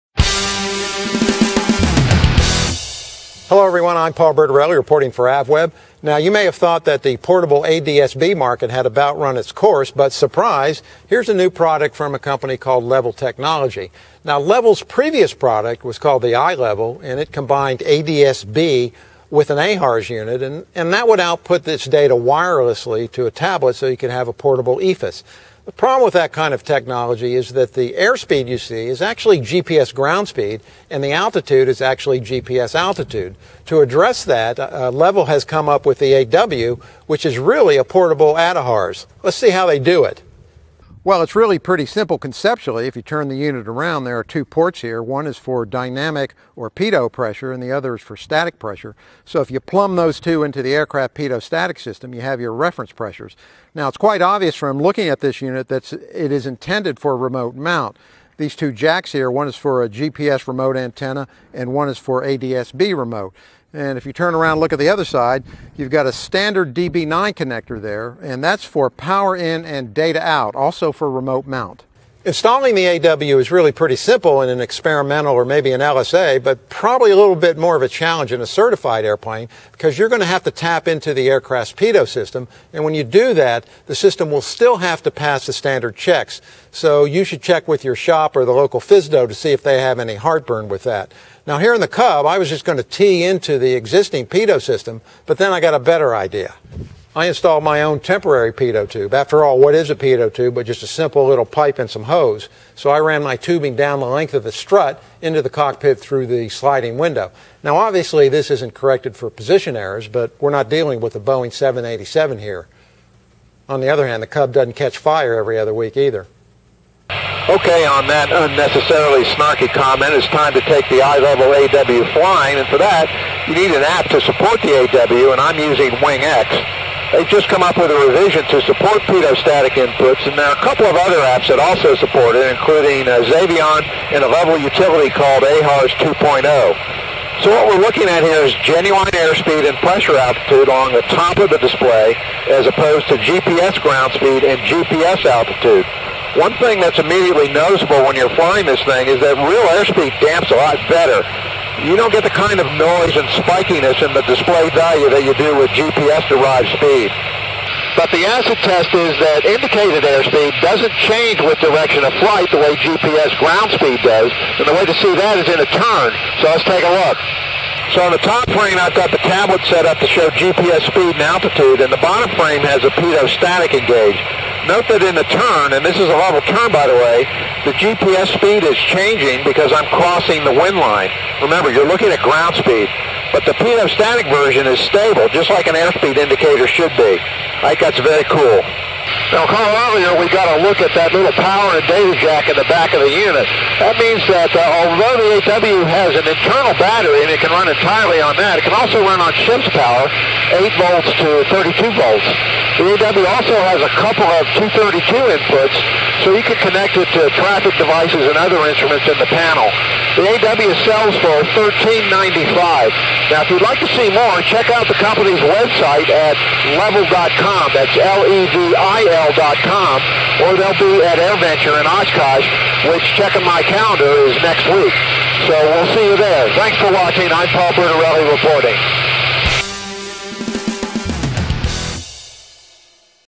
Levil Technology's New iLevilAW ADAHRS
Levil Technology is out with a new portable ADS-B unit that's really a miniature ADAHRS. Plumb it into your pitot/static system and it will wirelessly communicate attitude information along with indicated airspeed and pressure altitude to your tablet computer.